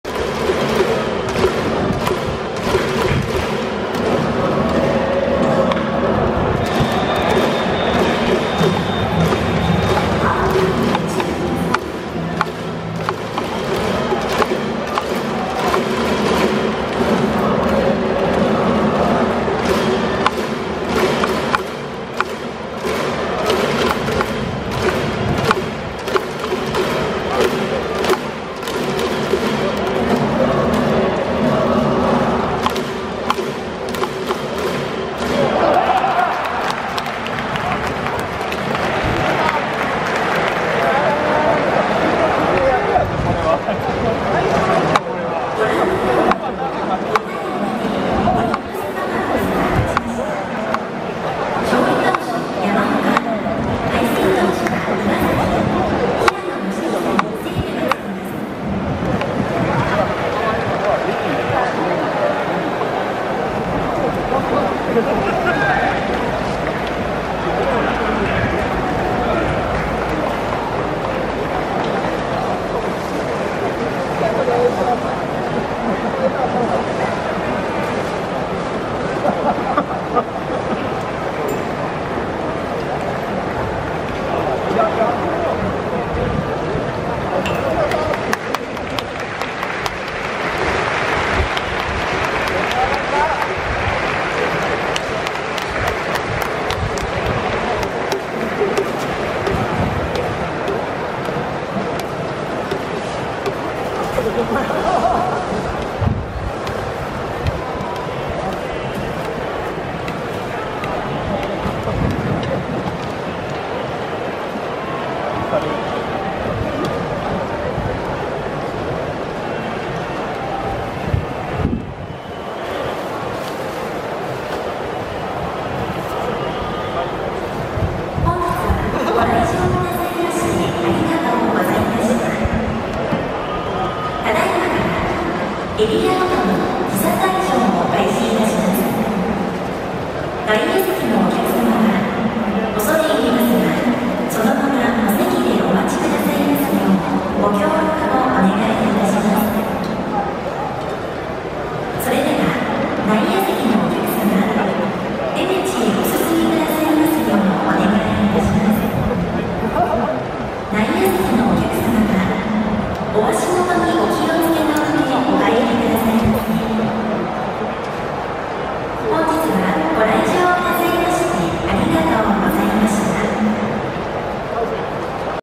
ネクストのロハス｡試合終了の瞬間 2021-6-1 オリックス戦
打者陽川尚将。ロハスがネクストで待機も、陽川尚将が倒れ試合終了の瞬間。ロハスの打席見たかったな！
明日がんばれ、タイガース！

マルテ9号ホームラン！井上ヘッド､佐藤､中野ベンチみんなでラパンパラ！ベンチが明るい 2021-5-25